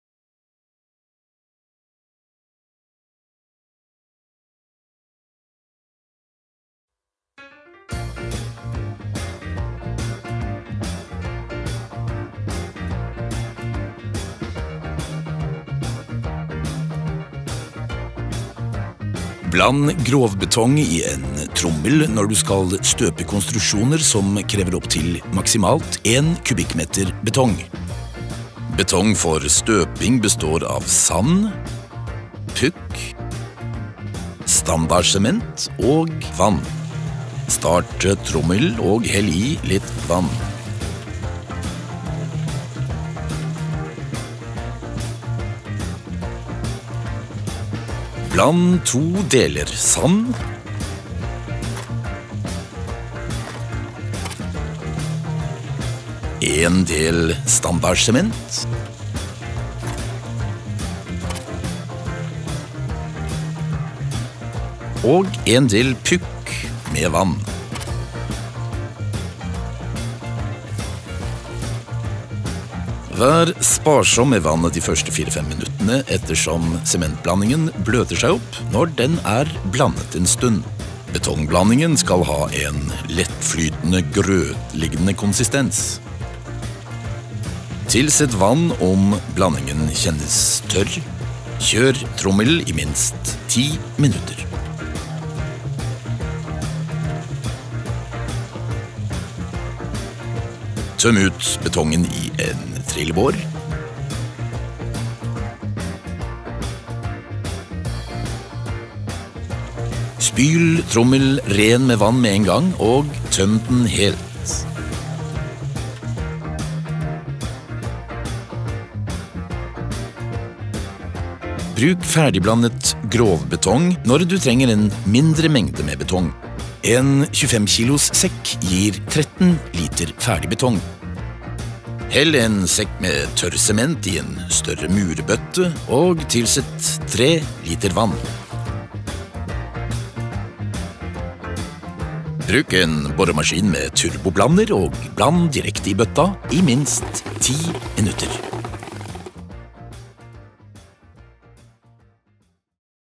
Byggmax tips, Blande betong i trommel eller bøtte (Del 1)
Vi viser hvordan du blander betong for støping. Bruk standarsement eller ferdigblandet betong!